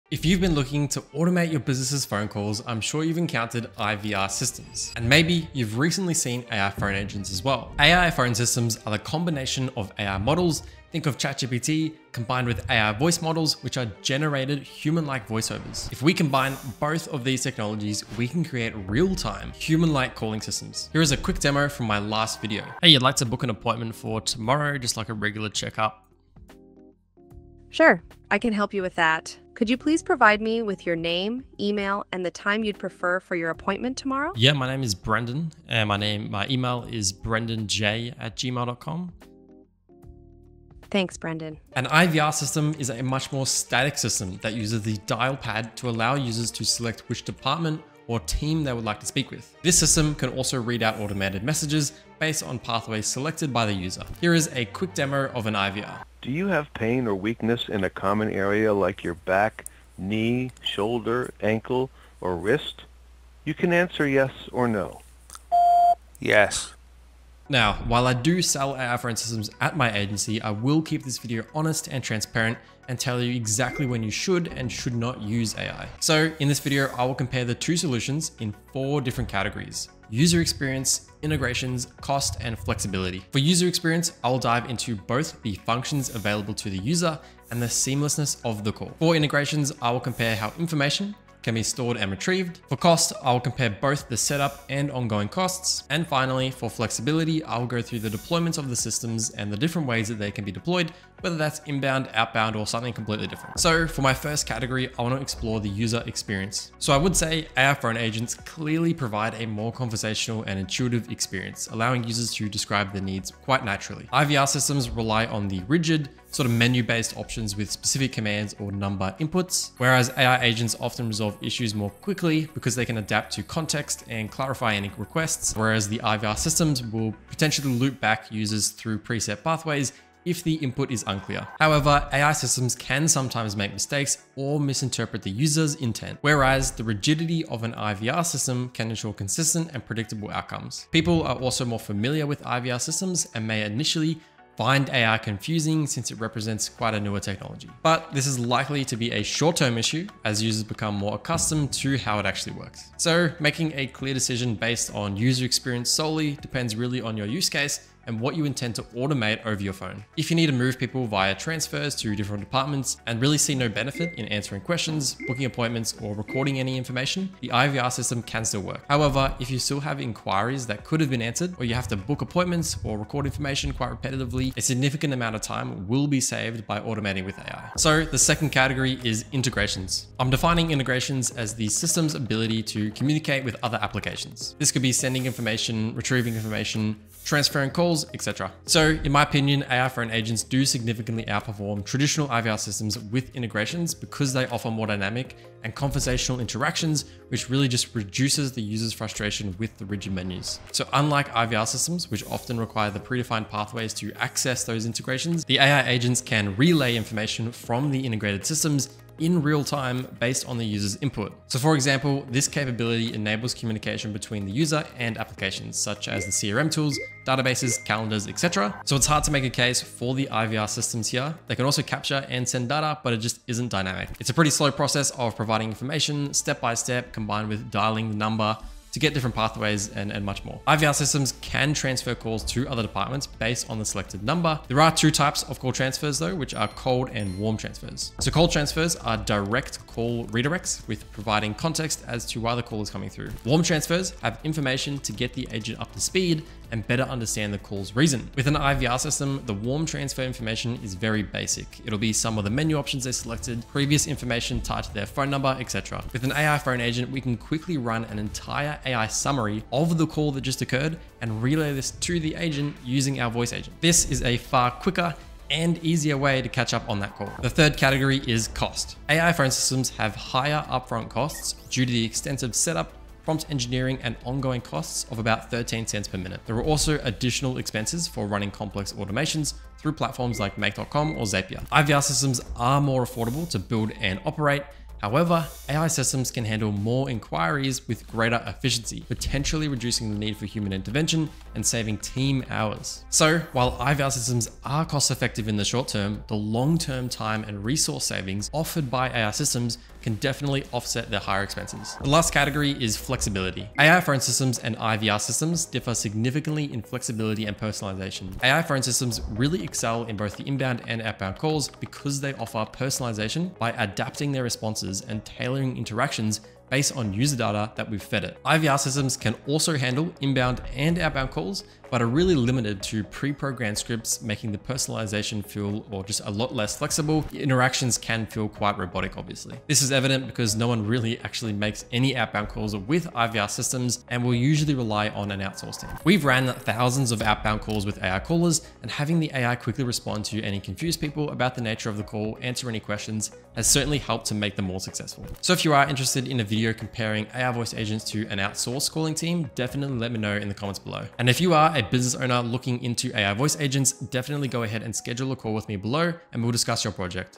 AI Voice Agents vs IVR Phone Systems
In this video, I compare IVR Systems to AI Phone Agents. Watch until the end to determine which phone automation method is best for your business. AI Phone Systems are the combination of AI models, think of ChatGPT, combined with AI Voice Models, which are AI generated human like voice overs. If we combine both of these technologies, we can create real-time human-like calling systems. An IVR system is a much more static system that uses the dial pad to allow users to select which department or team they would like to speak with. This system can also read out automated messages based on pathways selected by the user.

✅ Includes detailed responses to all your comments!

Chapters:
0:00 Intro
1:44 User Experience
2:57 Integrations
4:27 Cost
5:01 Flexibility